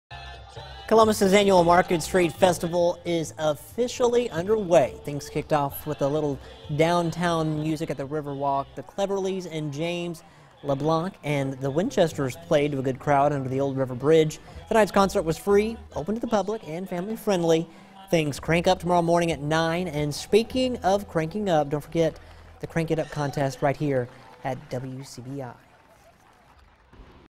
Market Street 05-03-19
Columbus annual Market Street Festival is officially underway that kicks off with a little music down at the riverwalk.